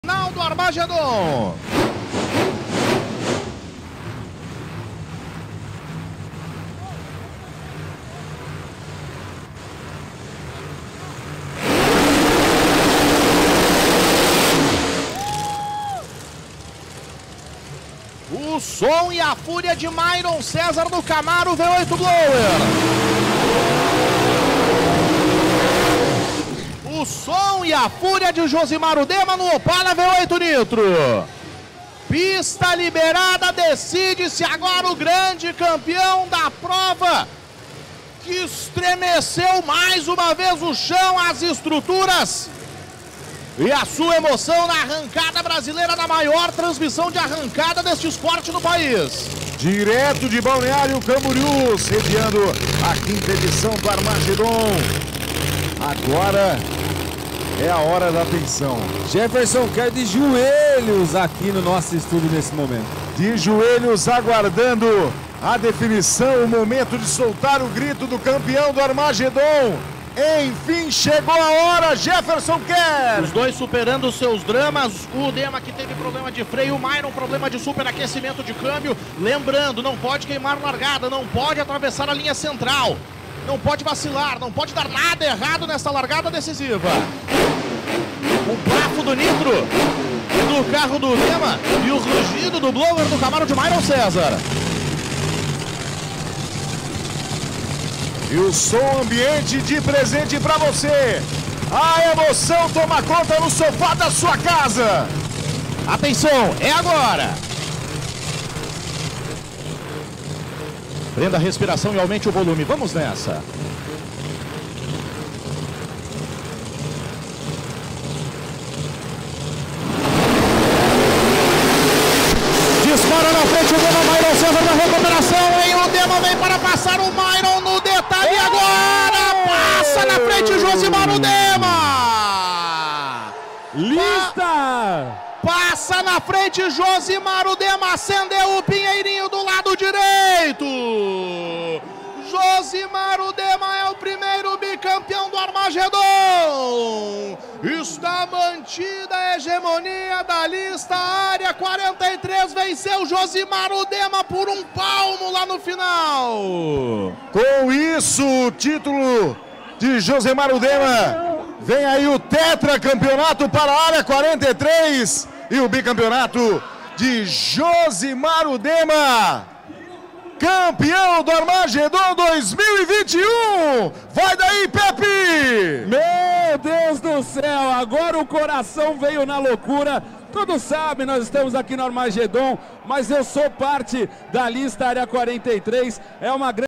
FINAL ARMAGEDON 2021 OPALA V8 NITRO Vs CAMARO V8 BLOWER
Reveja o trecho da Final do Armagedon 2021
Local: Speedway Music Park
Balneário Camburiu
Sábado 17 / Julho / 2021

Pilotos:
Área 43
Hudema: Opala V8 Aspirado Nitro

Área 34
Maicon: Camaro V8 Blower